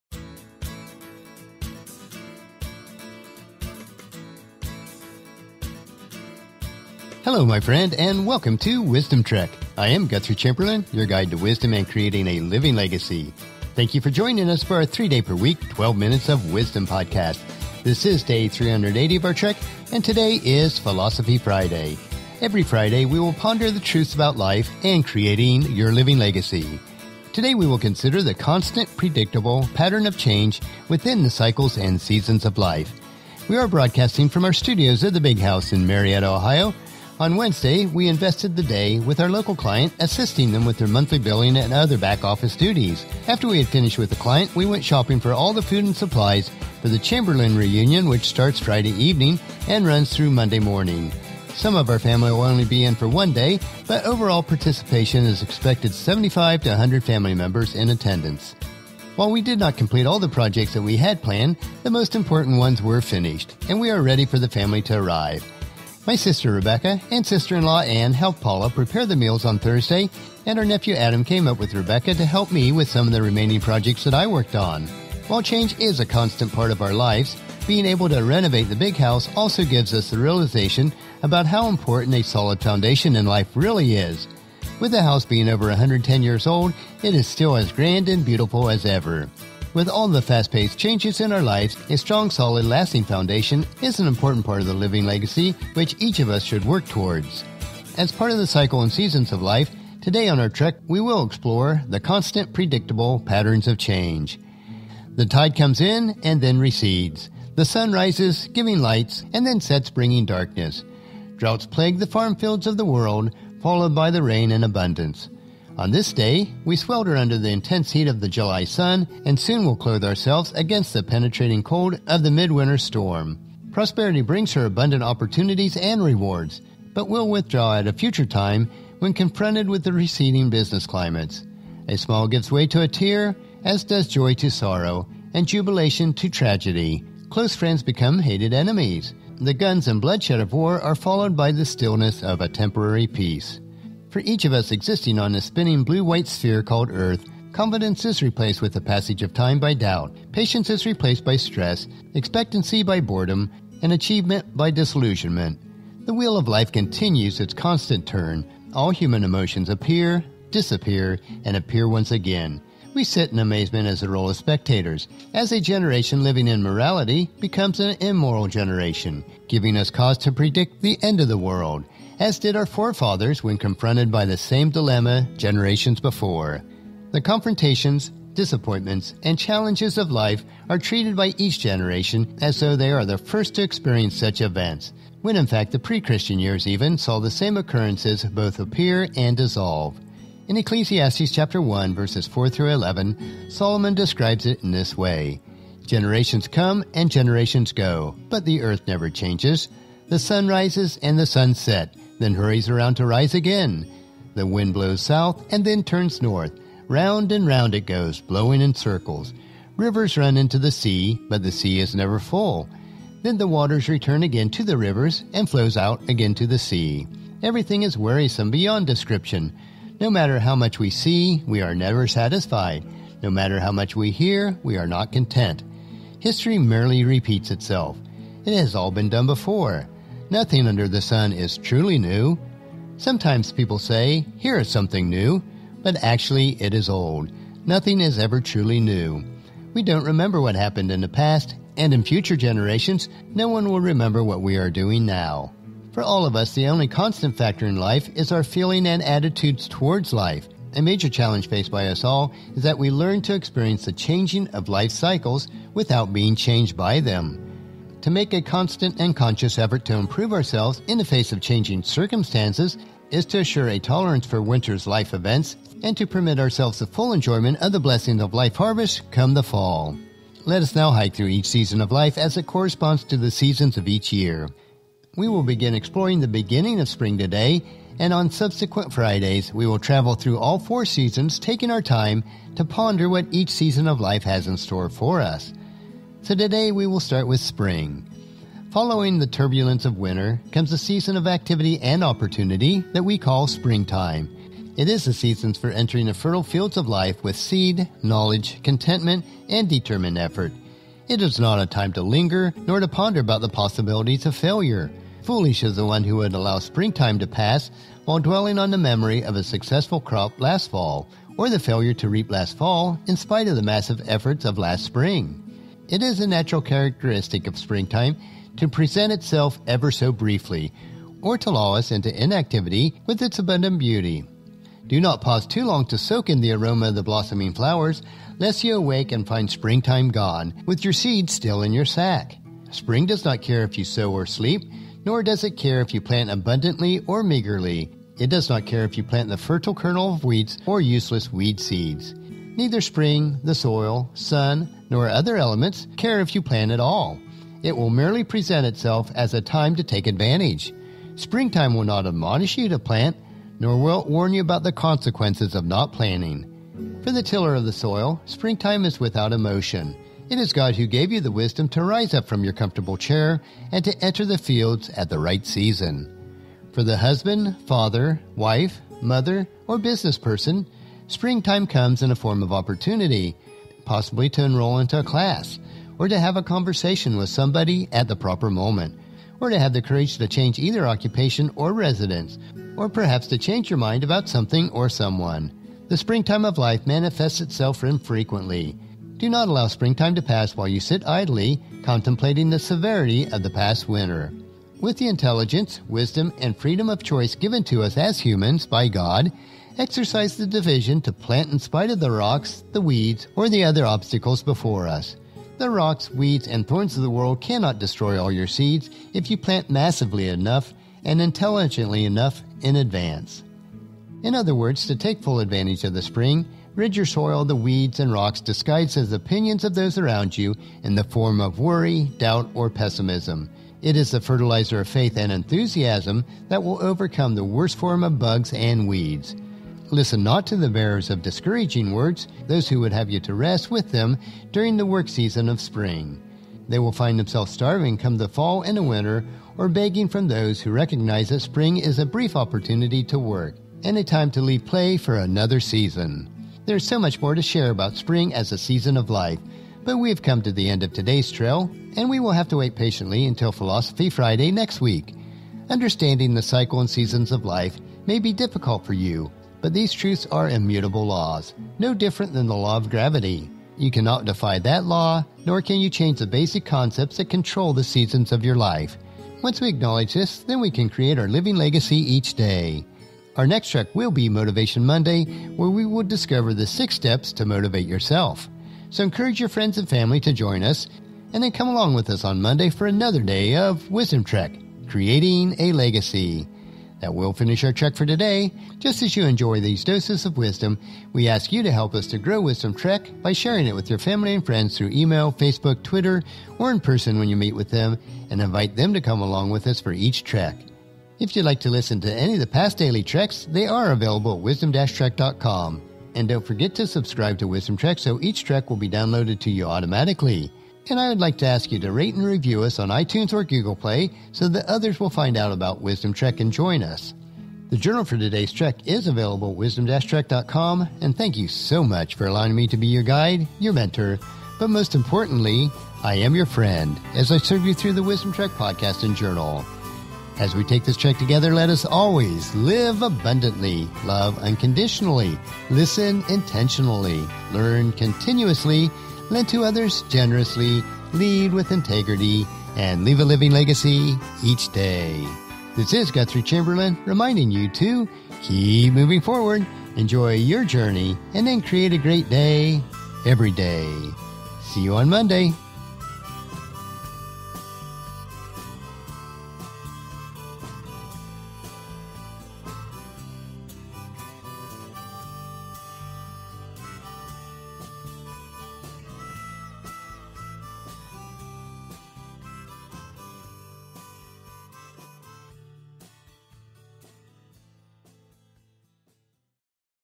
Day 380 – The Cycles and Seasons of Life (5) – The Awakening of Spring
As part of the Cycles and Seasons of Life, today on our trek we will explore change through…

The Constant, Predictable, Pattern of Change

The tide comes in and then recedes; the sun rises, giving light, and then sets, bringing darkness. Drought plagues the farm fields of the world, followed by rain in abundance. On this day, we swelter under the intense heat of the July sun, and soon we clothe ourselves against the penetrating cold of the mid-winter storm. Prosperity brings her abundant opportunity and rewards, but will withdraw at a future time when confronted by a receding business climate. The smile gives way to the tear, as does the joy to the sorrow and the jubilation to the tragedy. Close friends become hated enemies. The guns and bloodshed of war are followed by the stillness of a temporary peace